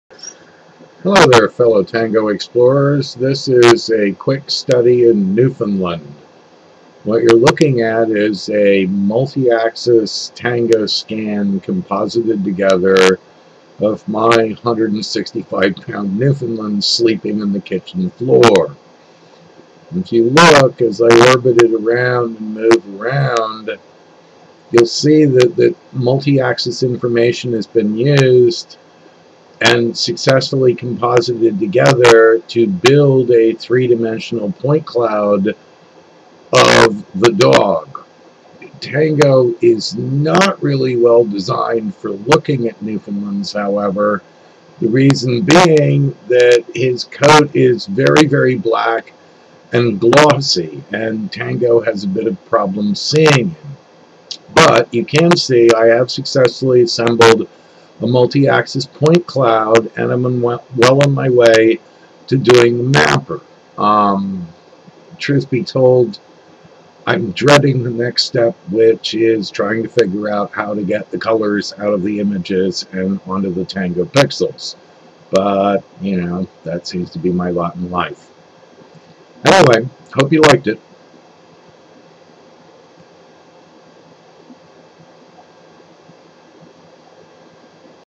Tango1 - Very first rough cut of multi axis tango data compositing
This is a video of data from a Google Project Tango tablet.  It shows the reassembly of multiaxis 3D scan data of my Newfoundland puppy dog, Pippin Ignatius Reilly. Tango is a way for your mobile devices to see the same world you do.